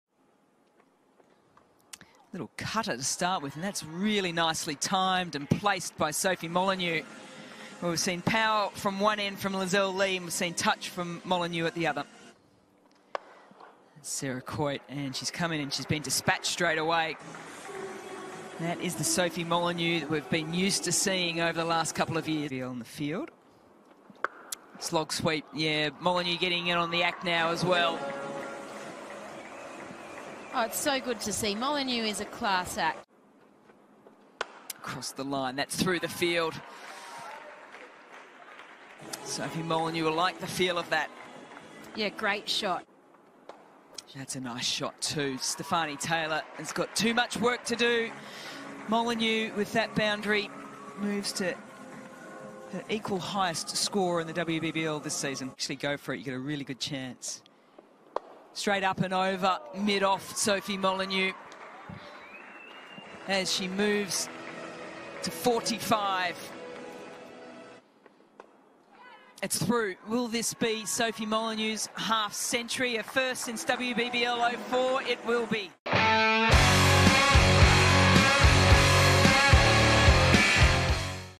Molineux posts classy fifty to push 'Gades to victory | Rebel WBBL|06 | Dream 11 MVP
Melbourne Renegades allrounder Sophie Molineux celebrated her first half-century of WBBL|06, posting 52 from from 43 to carry her side to the brink of victory